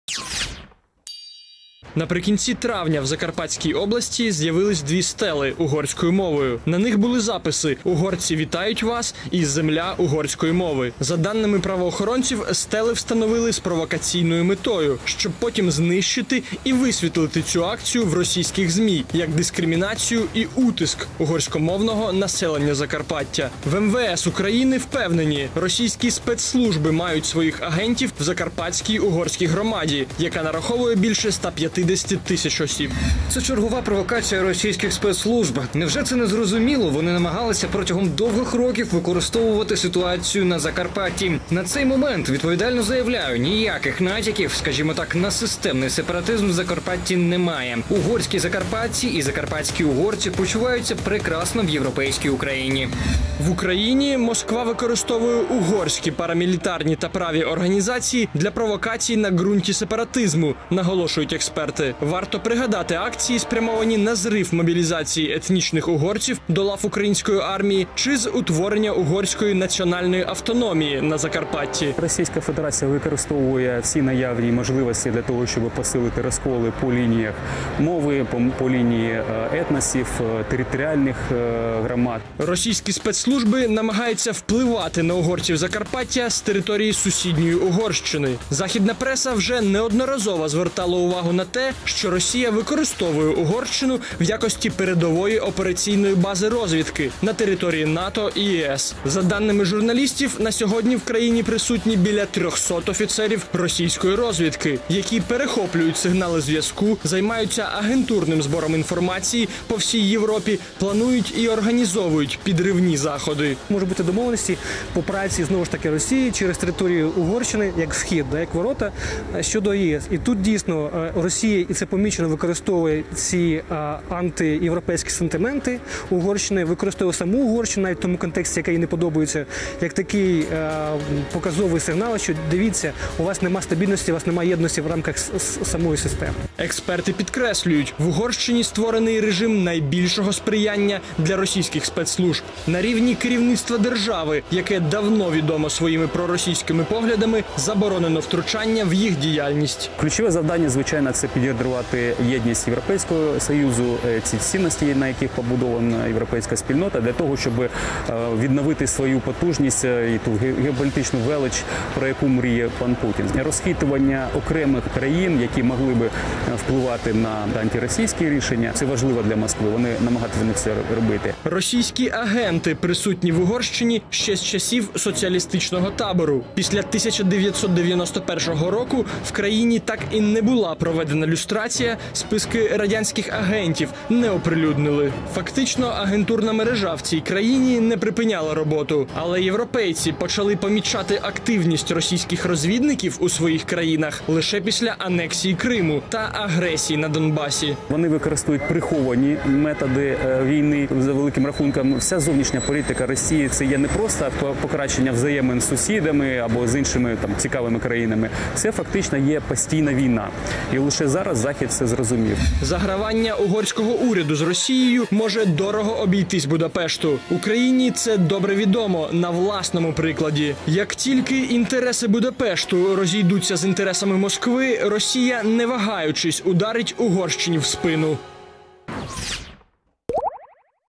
Россия использует Венгрию для сепаратистских провокаций в Украине
В Венгрии, которая является членом ЕС и НАТО, постоянно находятся около 300 офицеров российской разведки, пишет западная пресса. Их активность несет прямую угрозу не только Европе, но и Украине. Отечественное МВД утверждает: российские спецслужбы активно работают и с венграми, которые живут в Закарпатье. Чем шпионское гнездо угрожает Европе и нашей стране?